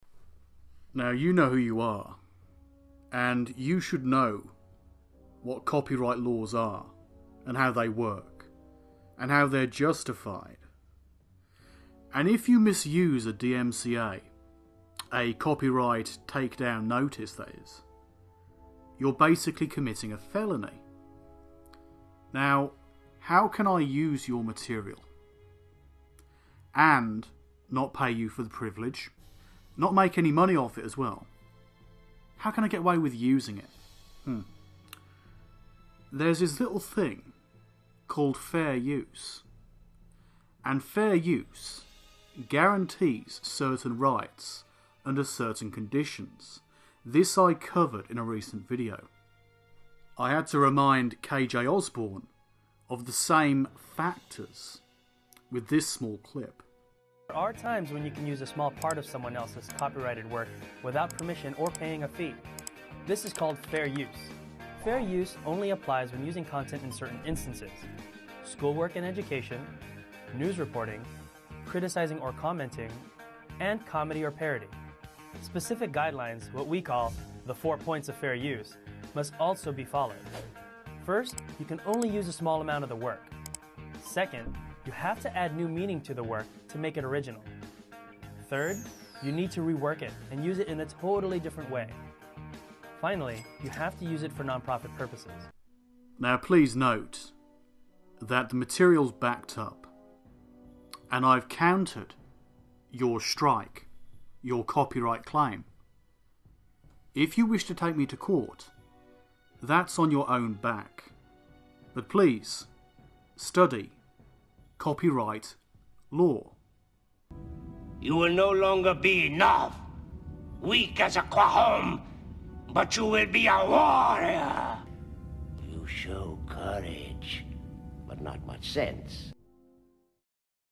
New Age Vs. Fair Use - Counter Claim Against Krista Raisa
You're in error, and I suspect you were using, or should I say misusing, the copyright system on youtube, and thus abuse the legality, to lower your levels of butthurt.

my video should be back up and running for comedy's sake in the next two weeks.